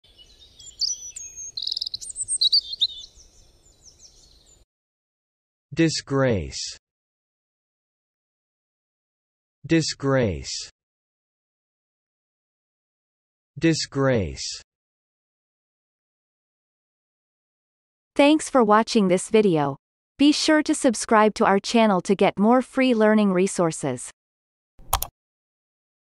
How to pronounce disgrace - Vocab Today
The pronunciation of the word disgrace in Amercian accent is demonstrated in this video. If the word is from another language, such as brand name, it will be pronounced in its original language. Based on the word frequency, this is of 3 stars difficulty.